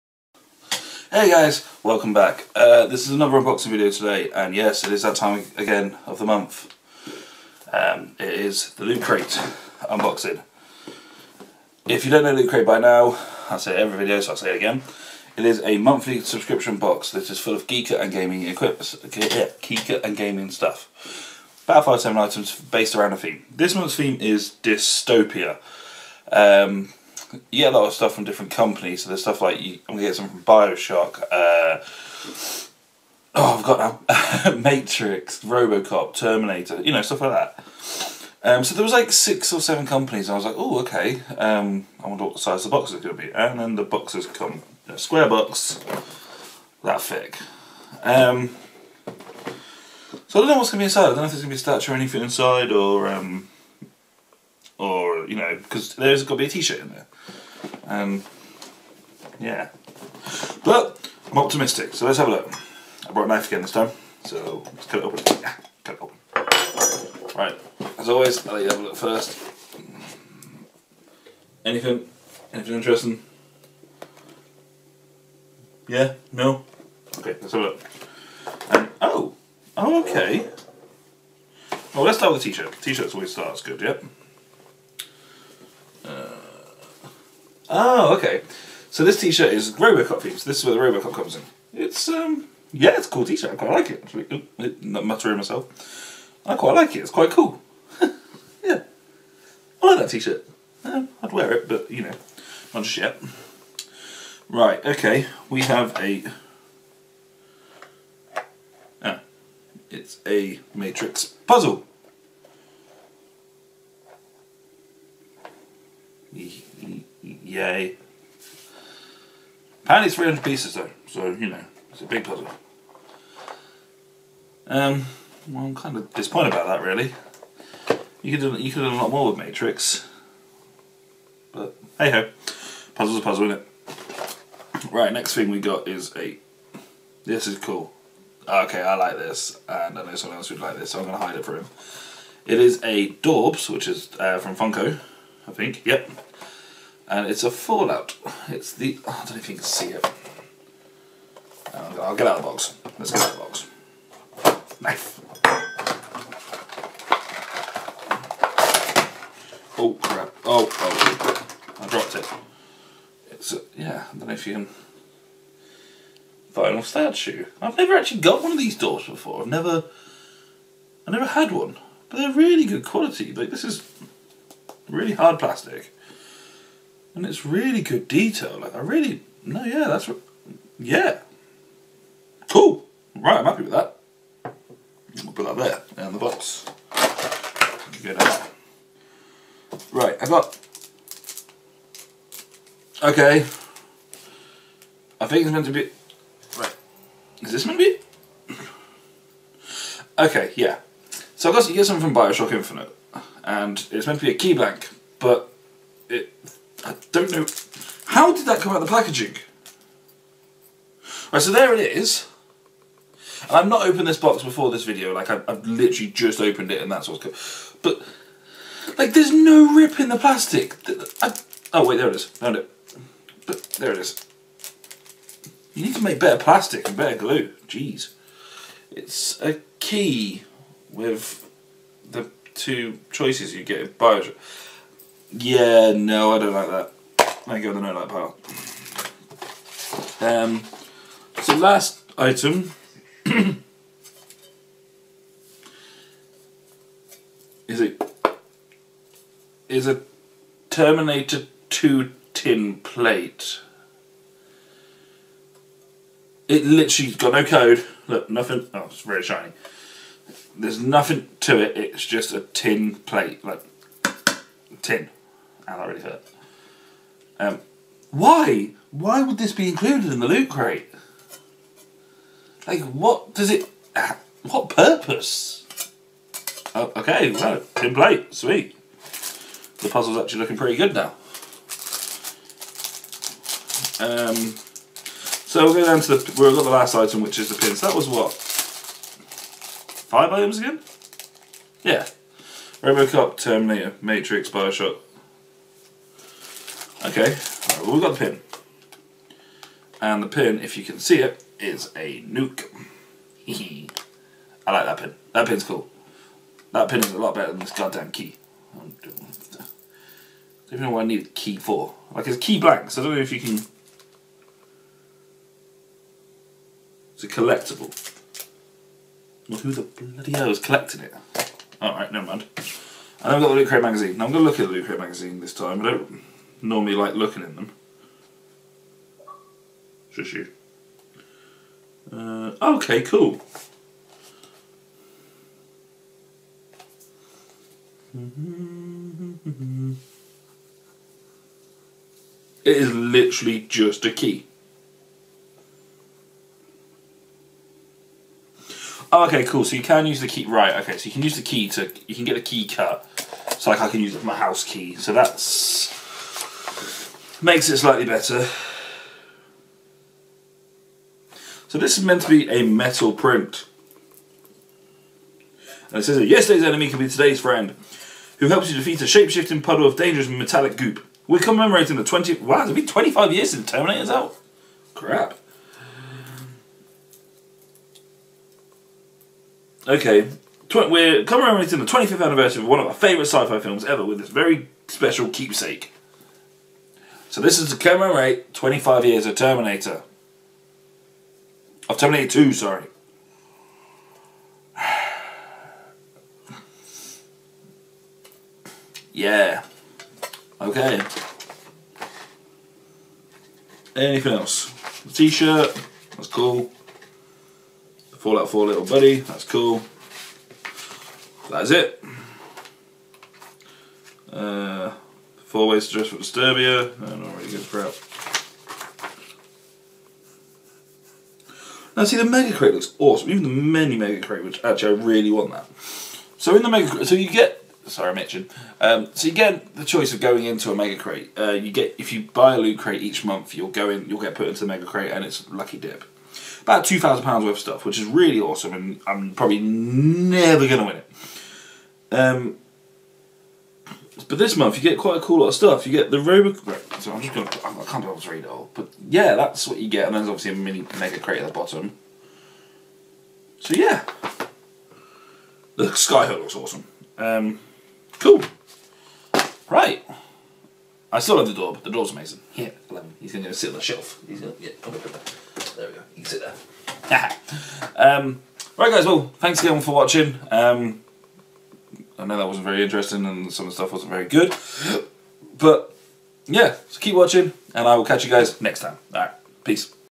unboxing time: lootcrate Dystopia (June 2016)
its unboxing time again boys and girls and this time it is the june lootcrate.